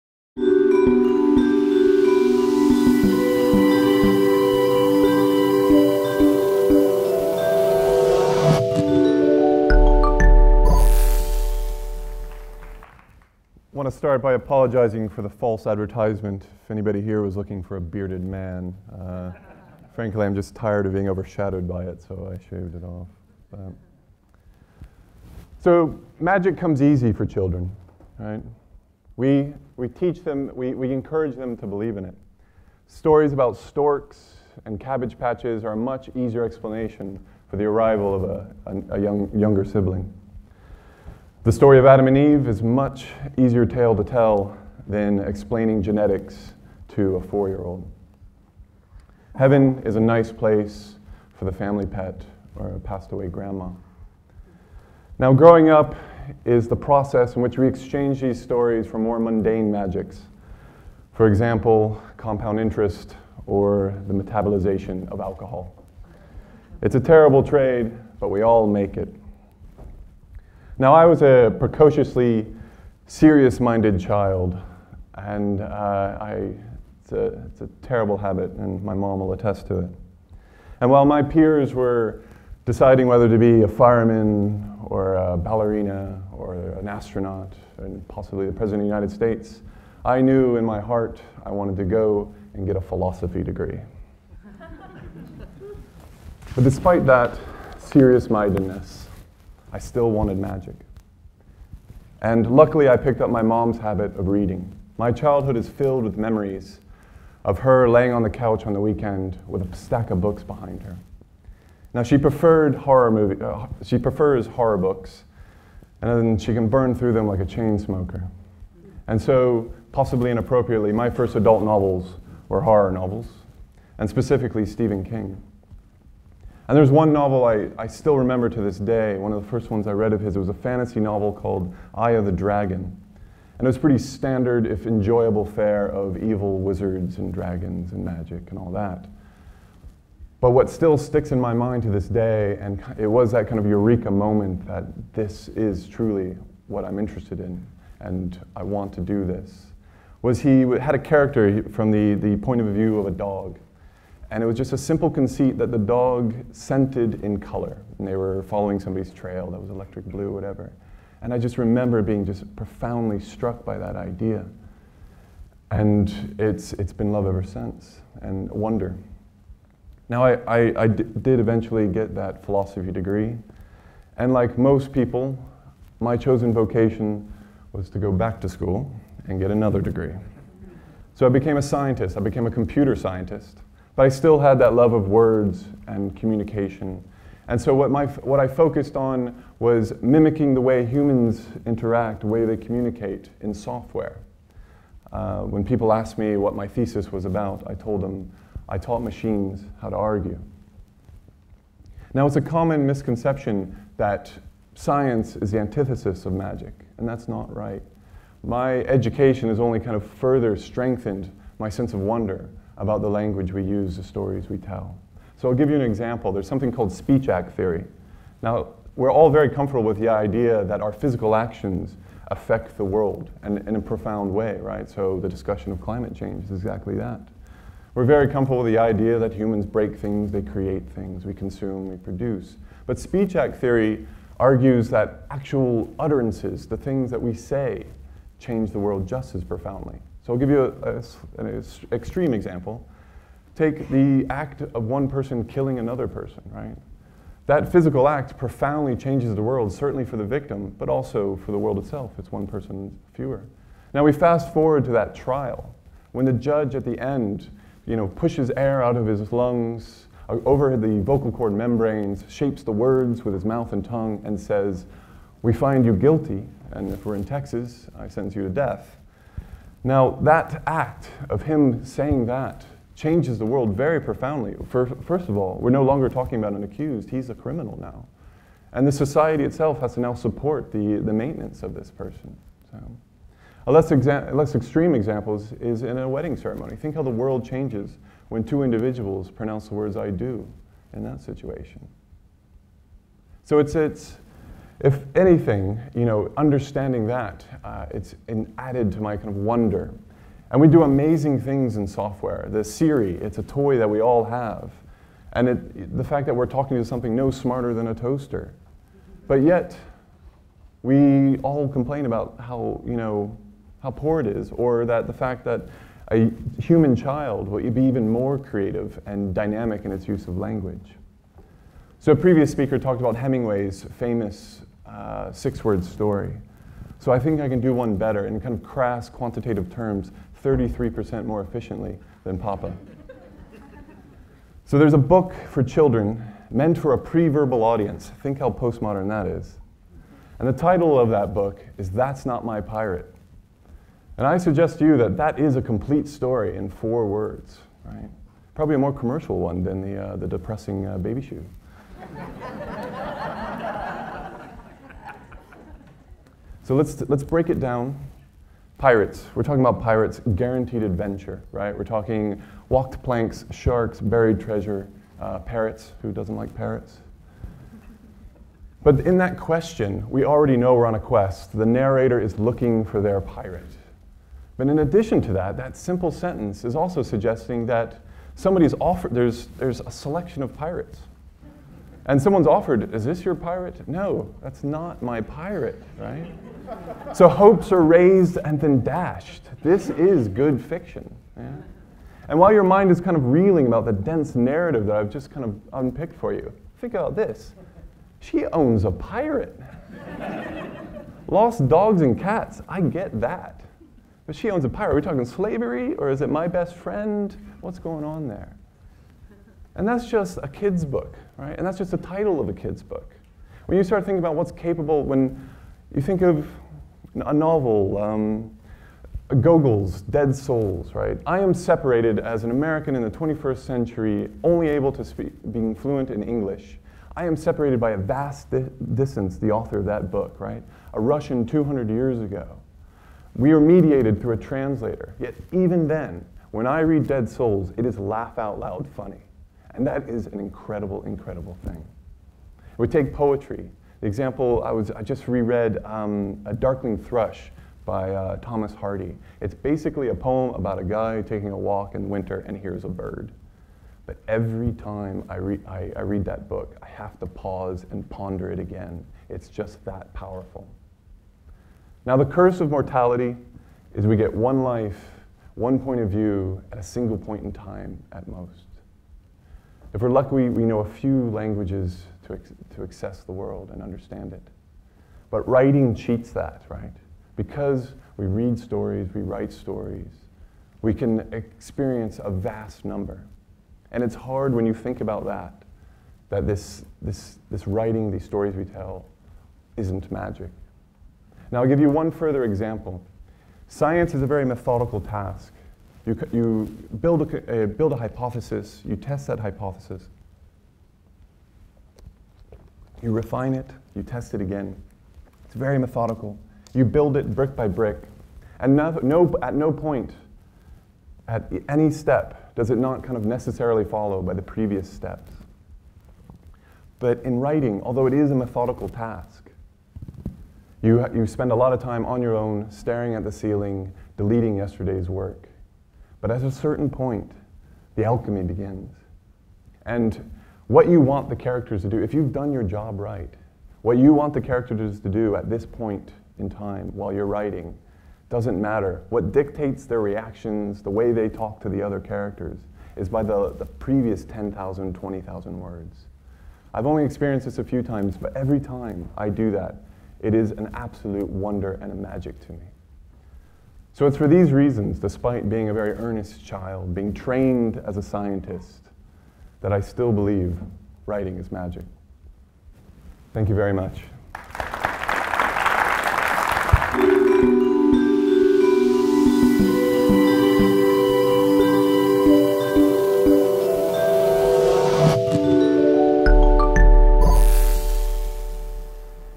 Writing is the only magic I still believe in: Jarred McGinnis at TEDxEaling
Jarred McGinnis will share his passion for stories and demonstrate the power of words from Speech Act Theory to the genius that is the children's book 'That's Not My Pirate'. Jarred is an American living in London, and the co-founder of the literary variety night, The Special Relationship. His fiction has been commissioned for BBC Radio 4, and appeared in journals in the UK, USA and Ireland. He is wickedtomocktheafflicted.com. In addition to writing fiction, he holds a PhD in Artificial Intelligence.